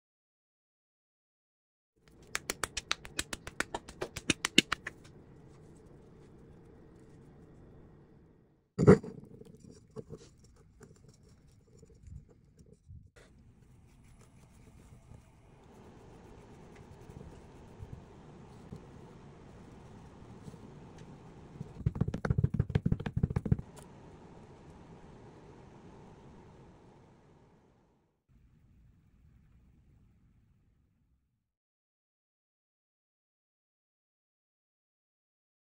Creating a Clay Ladle - Black on White Replica
Join us in this engaging journey as we shape and mold clay into a beautifully crafted ladle. This carefully crafted replica of a Roosevelt black on white ladle will be a lot of fun to make!